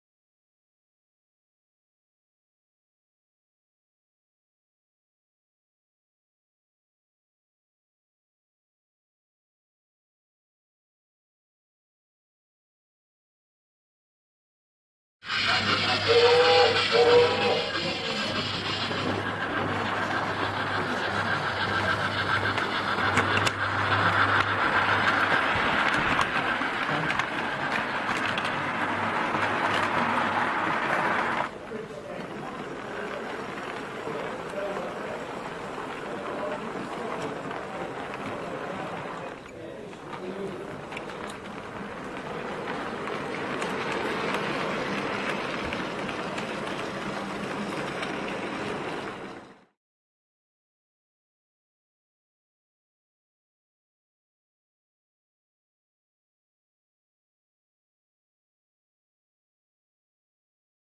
Elham Valley MRC 1923 Era
Film of Elham Valley railway modelling the period just after Grouping in 1923.
Filmed at Peene in Sept 2023